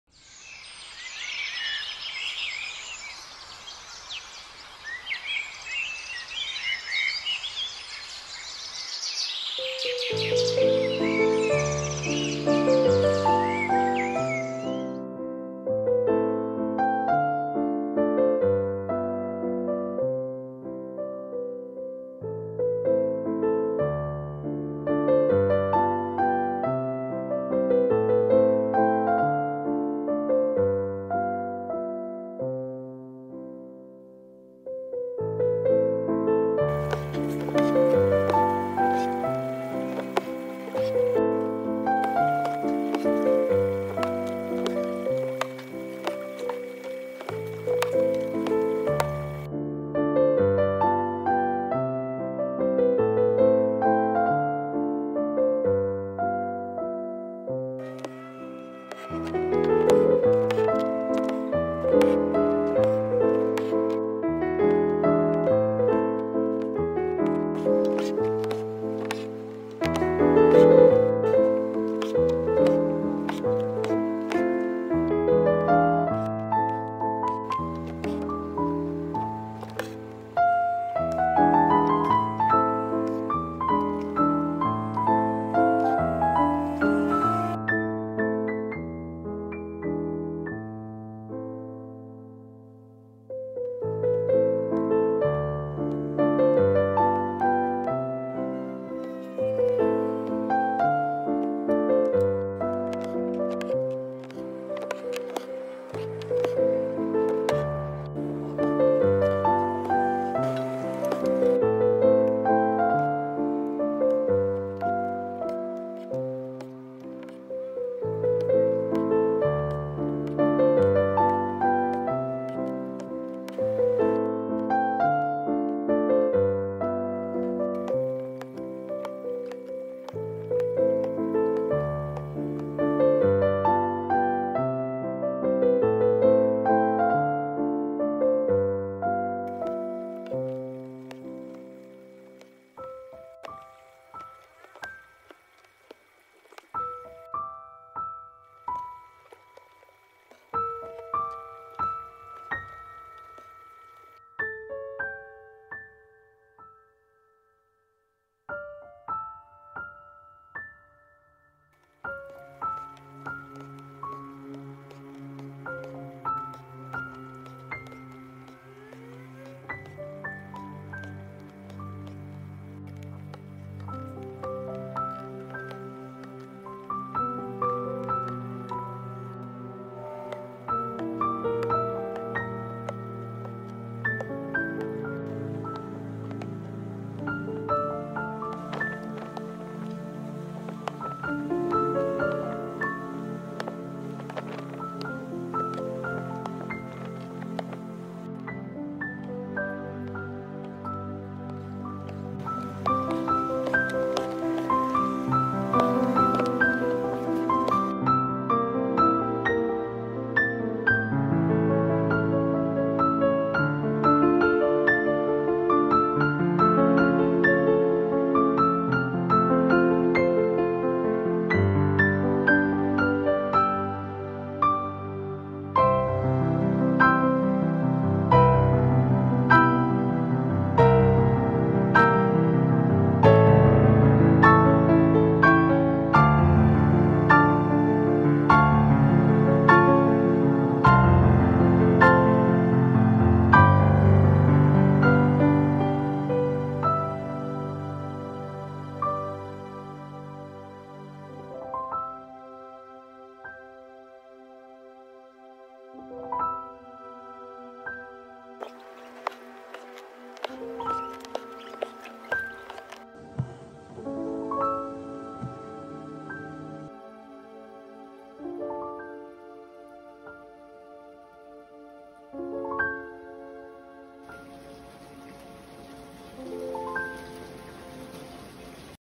**Hand Embroidery** Gardenia Flower Needle Painting Time Lapse
I used 1 strand only for this embroidery, mostly using long & short stitch and needle painting technique, but I regretted it so much after working on it for few hours.. I shouldn’t torture myself with that.. so many stitches just for one petal. Anyway, hope you enjoy the video, it took me 3 days to finish this embroidery.